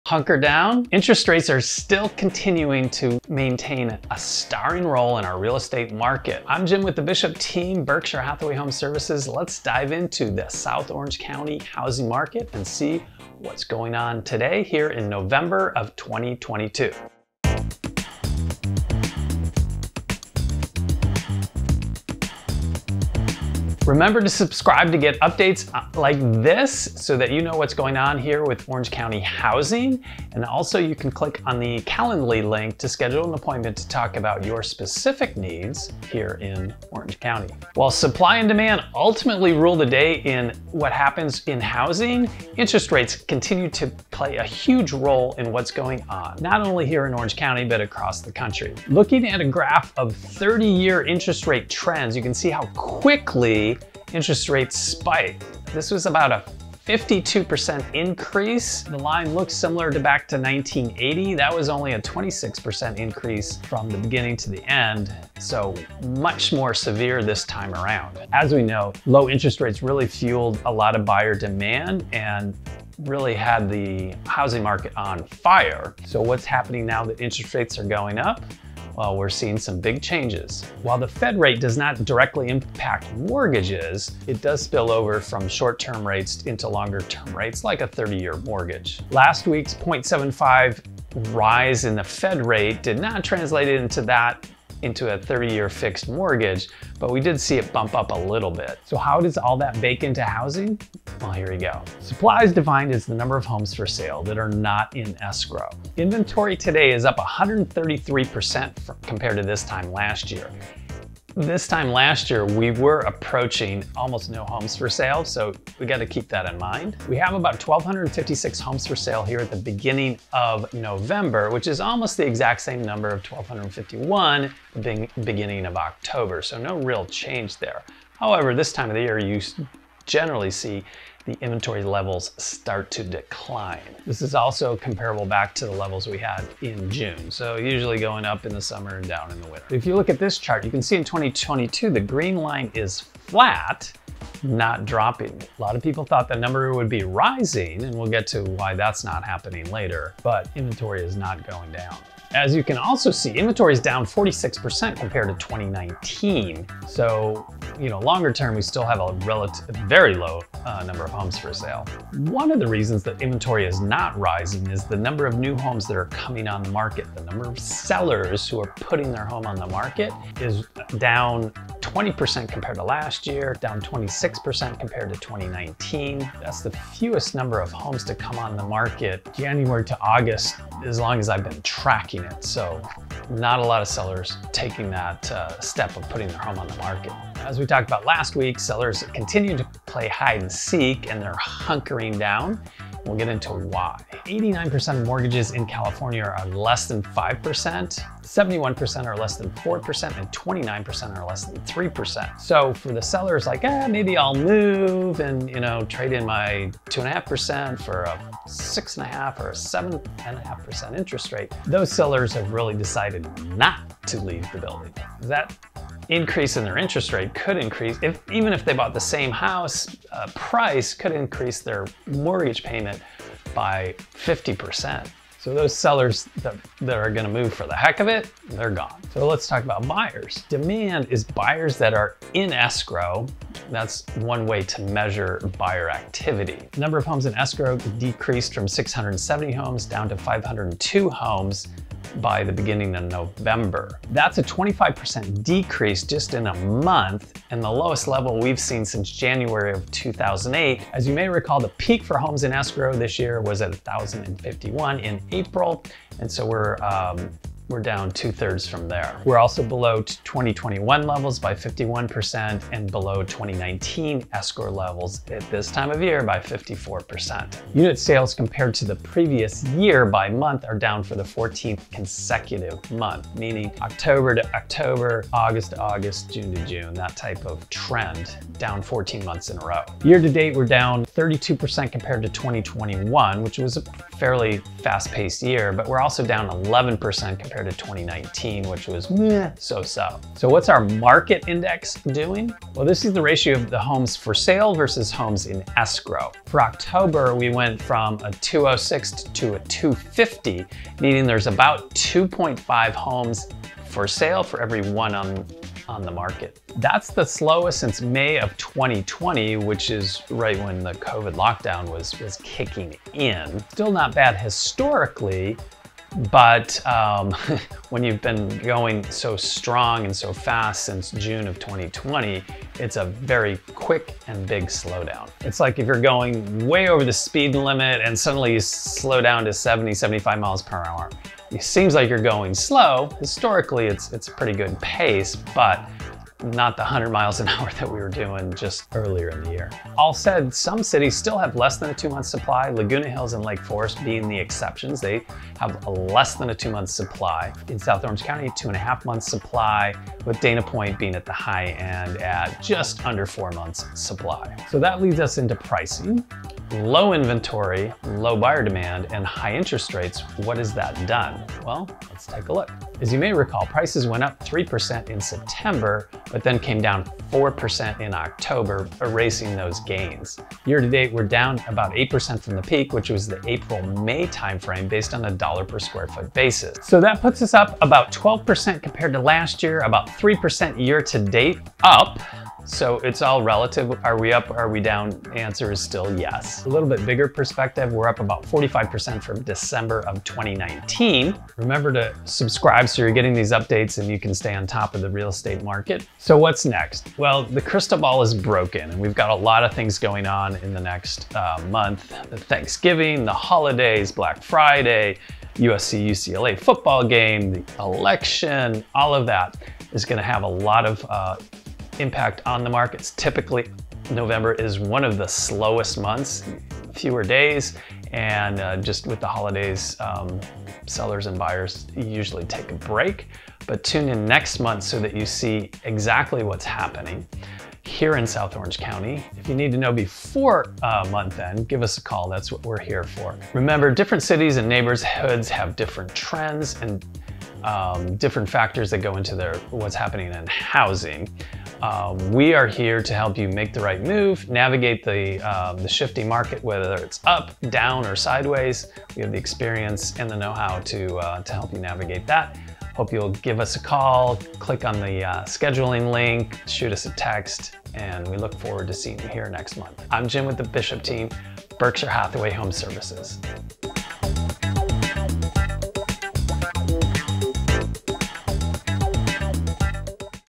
November 2022 Housing Market Update South Orange County by The Bishop Team
What's going on in the market? Have Interest Rates had an impact on Housing Prices?  What is Buyer activity like? Check this out for the latest in South Orange County Housing

Jim Bishop
Berkshire Hathaway HomeServices California Properties
DRE 01352560

Justin Bishop
DRE 02120138

Vickie Bishop
DRE 01982437